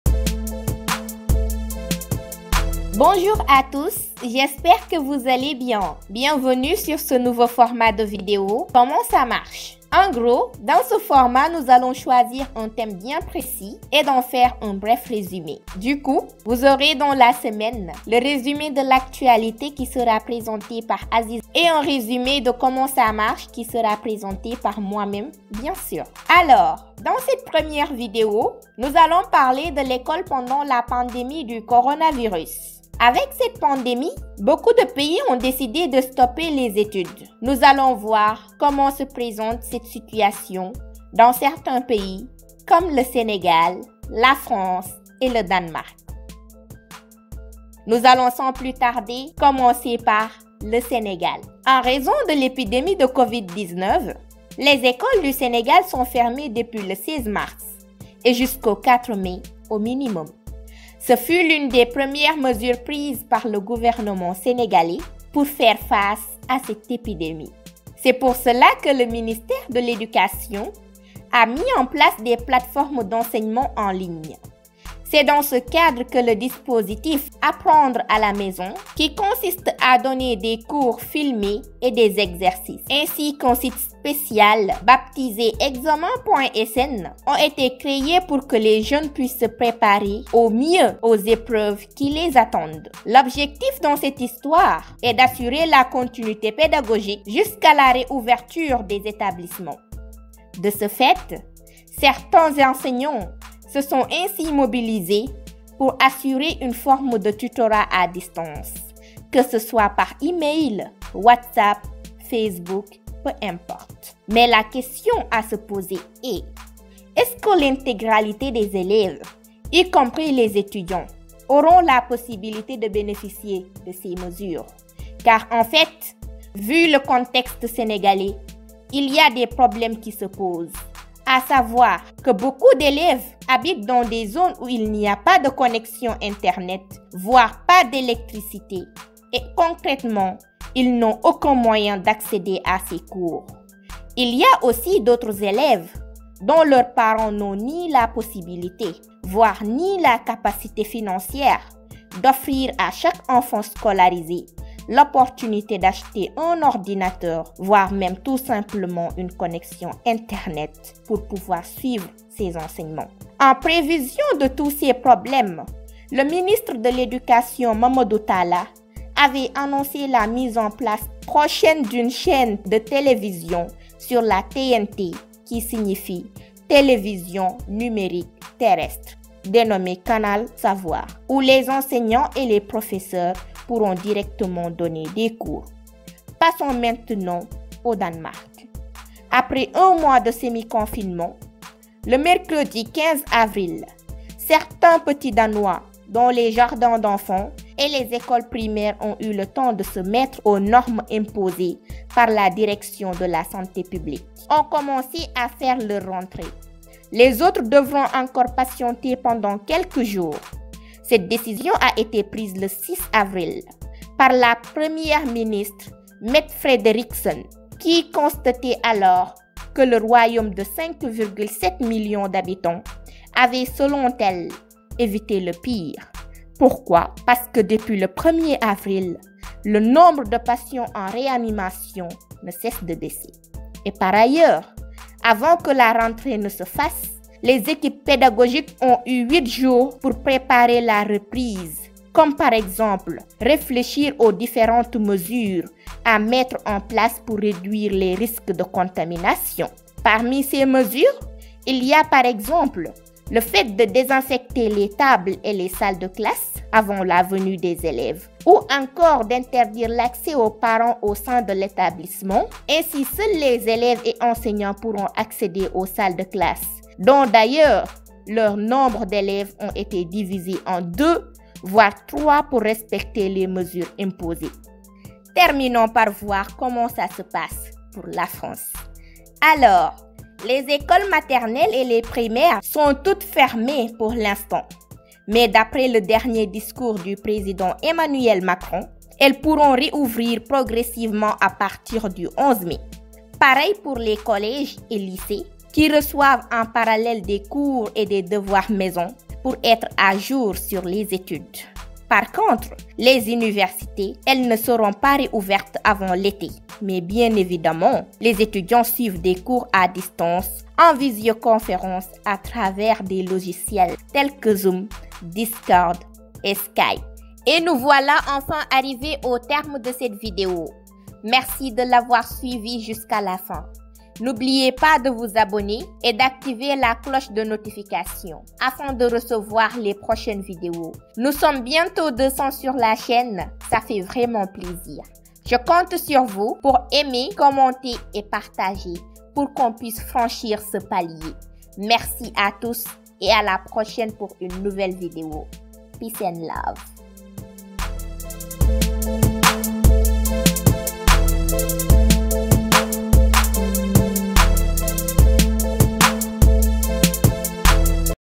Comment ça marche #1: l'Ecole pendant la pandémie de Covid-19
Avec cette pandémie beaucoup de pays ont décidé de stopper les études nous allons voir comment se présente cette situation dans certains pays comme le Sénégal, la France, le Danemark.

Sources